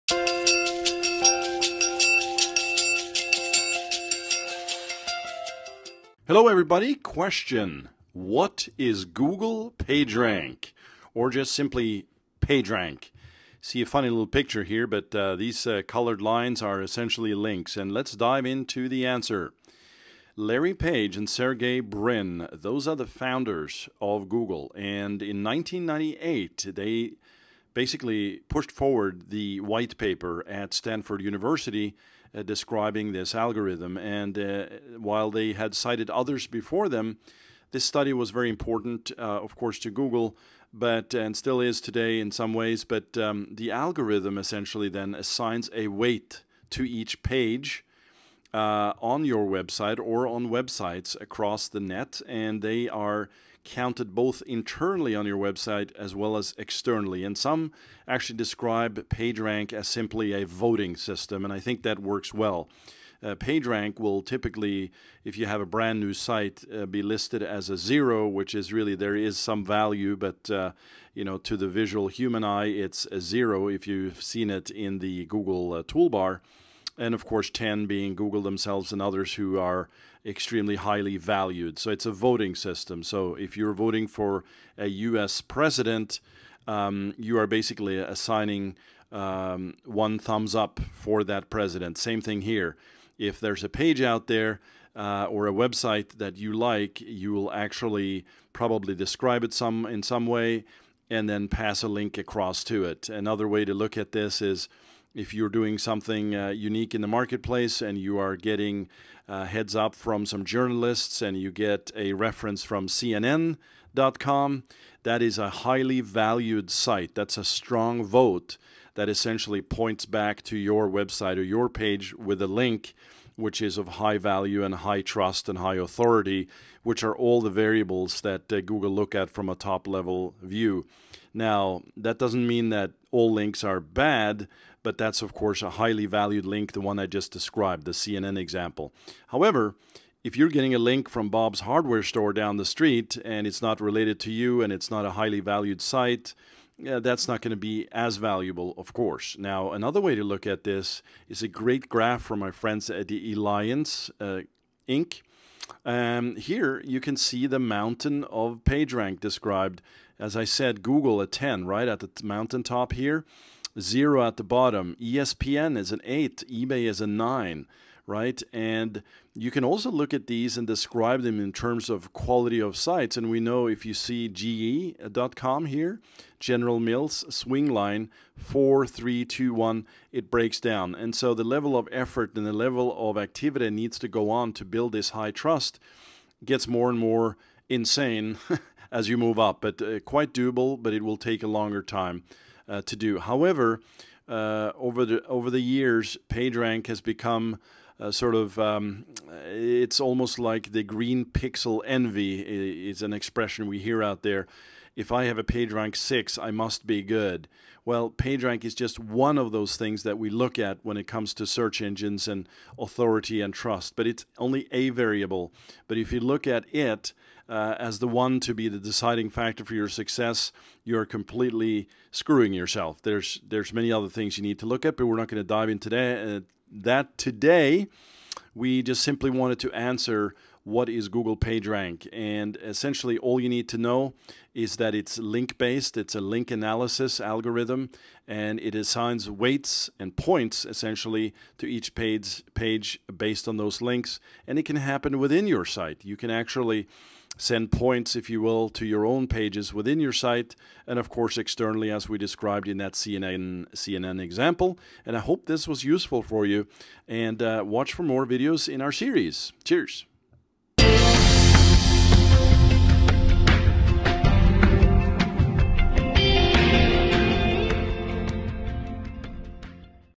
What is Google PageRank? (Google search engine)
- What is Google PageRank? How does Google calculate this core link algorithm that began the Google empire back in 1998?